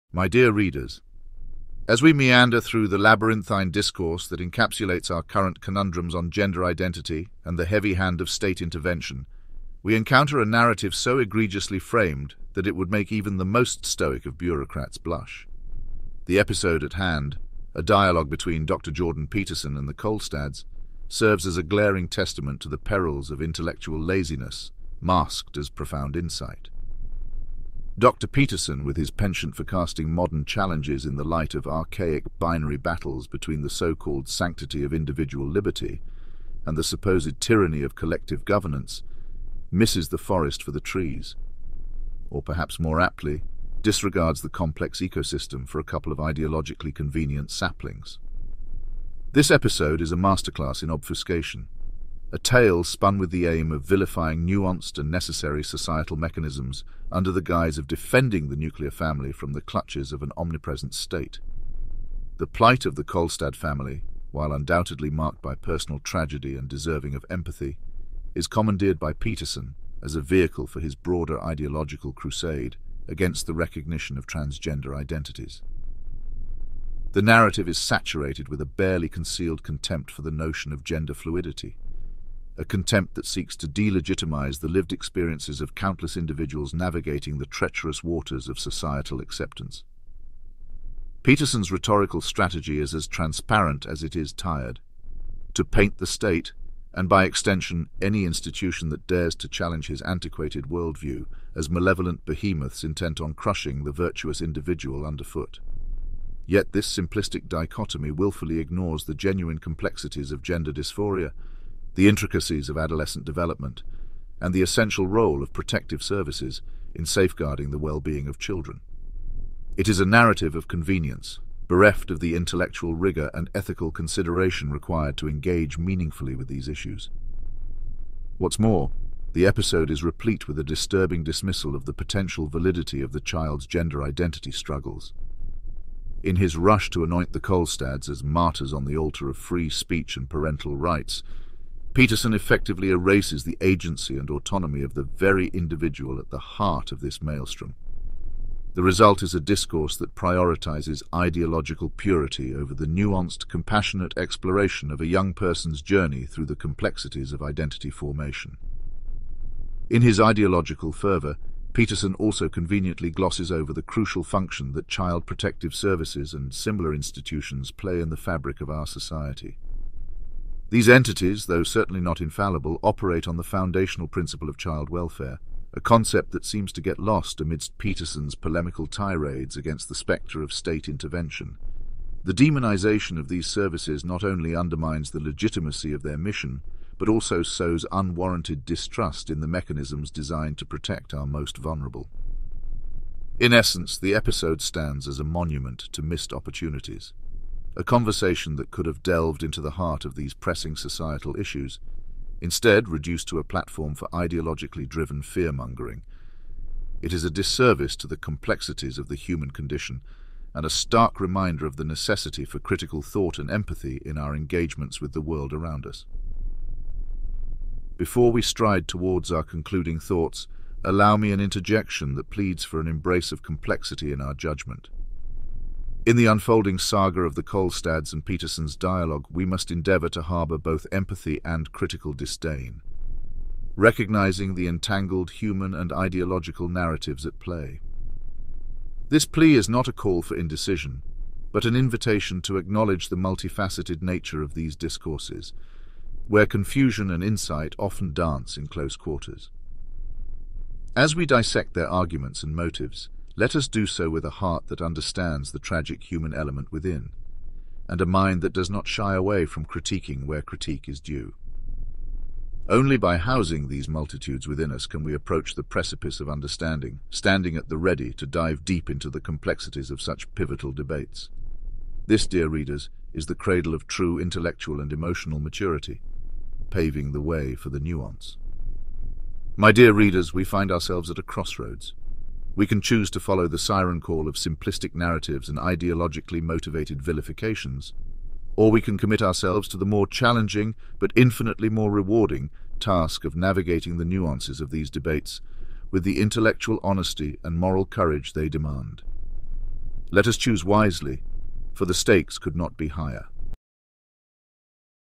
A.I. Christopher Hitchens on Jordan Peterson Ep.428
This video is an A.I. generated imagining of how Christopher Hitchens might sound discussing episode 428 of Jordan Peterson's podcast. The script was generated via Llama 2. This is an artistic, noncommercial parody, employing Christopher Hitchens' recognized style for lighthearted pop culture commentary. It's crafted for artistic expression, entertainment, and educational purposes.